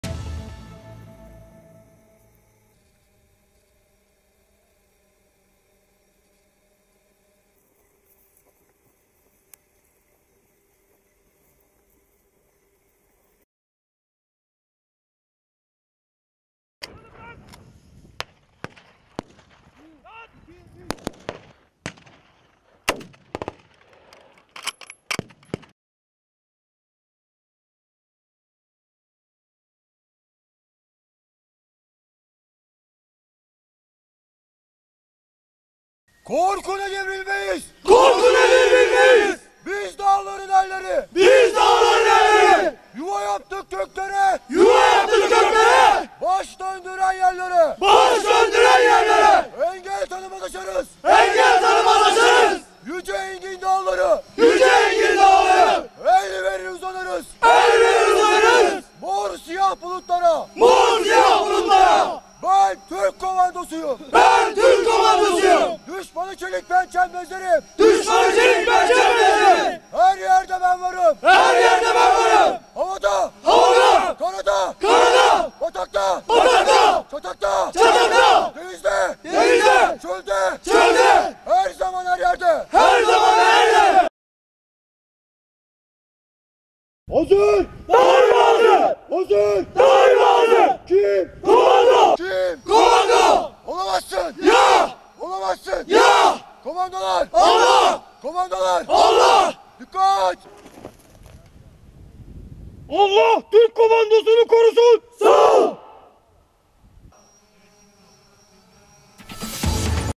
Dağ Komandoları farklı meslekten olsa da hedefleri aynı
Komandoların meslekleri farklı hedefleri vatan
Dünyanın en zorlu askeri eğitimlerinin verildiği Isparta Eğirdir Dağ Komando Okulunda Türkiye'nin farklı yerlerinden gelen asteğmenler, meslekleri farklı olsa da vatanı koruma hedefi doğrultusunda zorlu bir eğitime tabi tutuluyor.